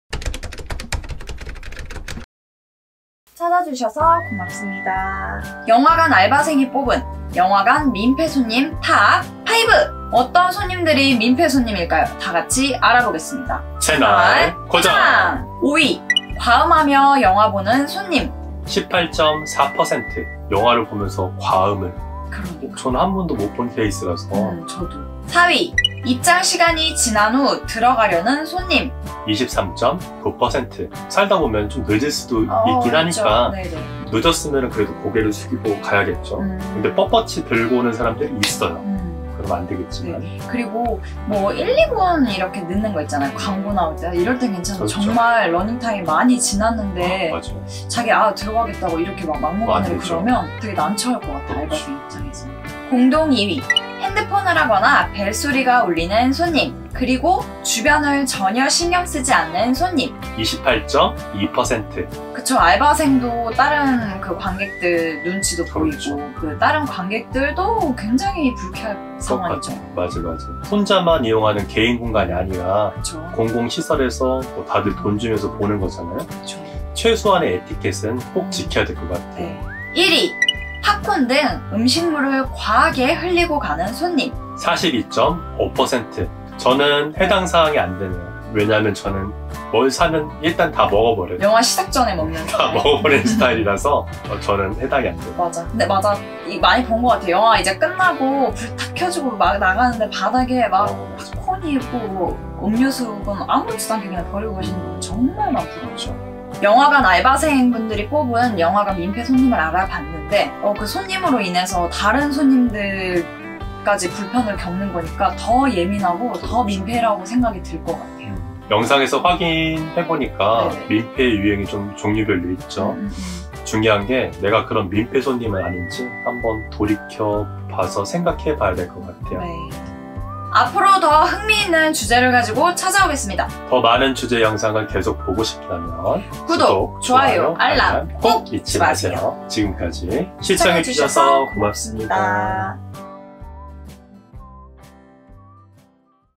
영화관알바생이 뽑은 영화관 민폐손님 탑5
어떤 사람들이 민폐를 줄까?!
나는 혹시 그런 손님이 아닌지 확인해보세요!